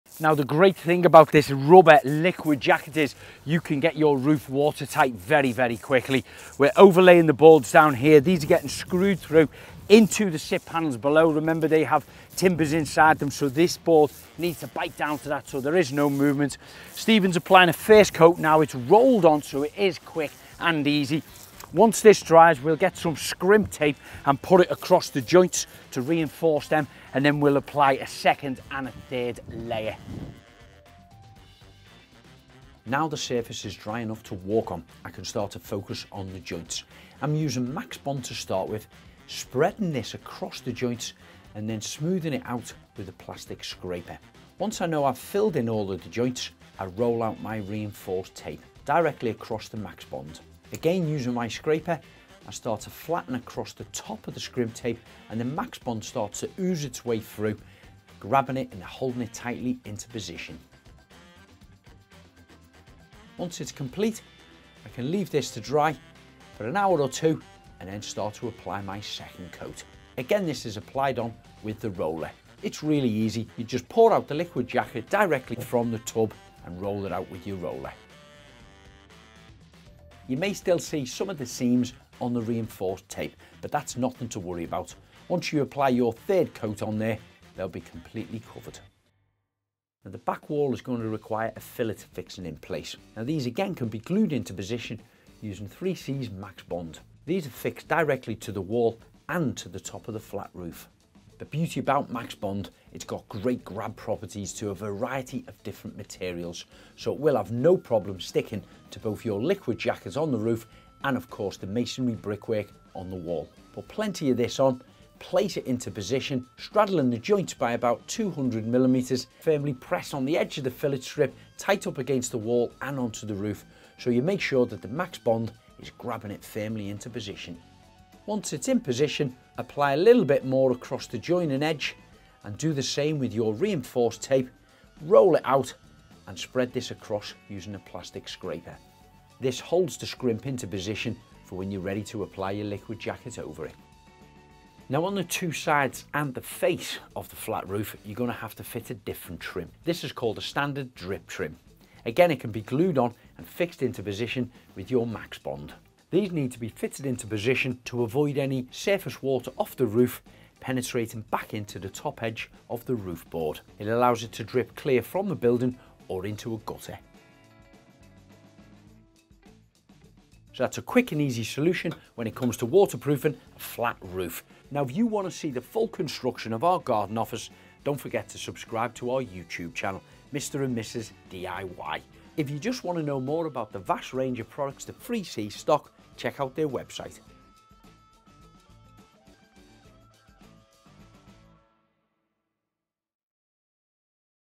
Sealing A Flat Roof With Liquid Jacket Liquid Rubber Membrane
In this video, 3C Sealants' brand ambassador, Craig Phillips demonstrates how to waterproof and seal a flat roof with Liquid Jacket Liquid Rubber Waterproof Membrane.

Liquid Jacket Rubber Waterproof Membrane is a ready to use, highly elastic, modified polymer sealer that can be applied in dry and damp conditions.

Supplied as a one-part, ready-to-use thick liquid, it can be applied by brush, trowel, squeeqee, scraper or spatula. This highly versatile rubber membrane instantly waterproofs practically anything and is especially useful for sealing, repairing, coating and waterpoofing walls, roofs and floors, new and old!